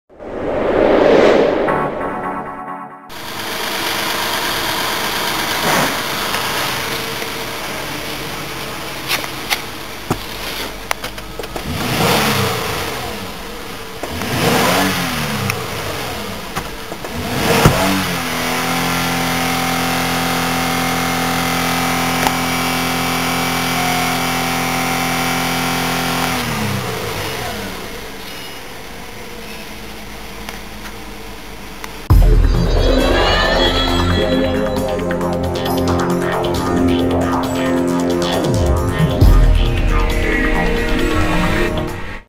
WRECKING 2016 KIA CARNIVAL/GRAND CARNIVAL 3.3 AUTOMATIC (C24787)
We are currently wrecking this 2016 KIA CARNIVAL/GRAND CARNIVAL 3.3 AUTOMATIC (C24787).